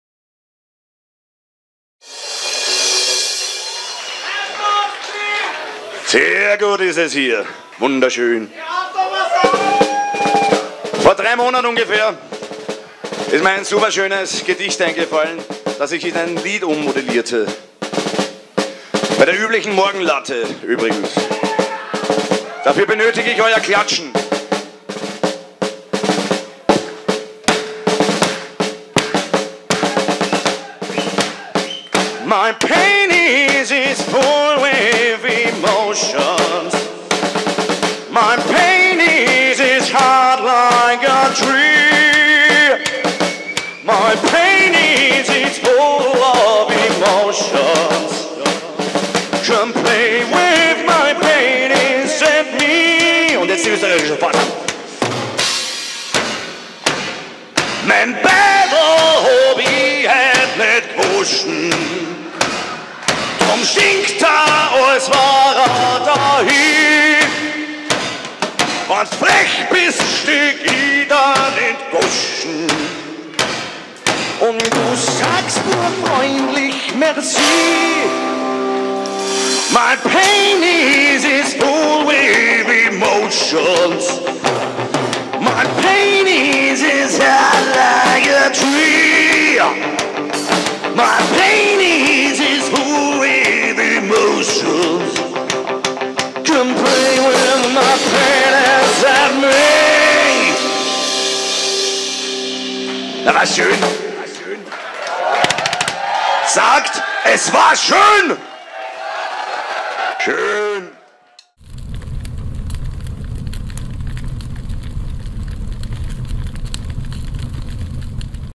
Magnus Anus/Live in der Szene
Livemitschnitt vom 16.10.09 in der Szene.Magnus Anus ist an diesen
Abend die Vorband von JBO.War ein geiles Konzert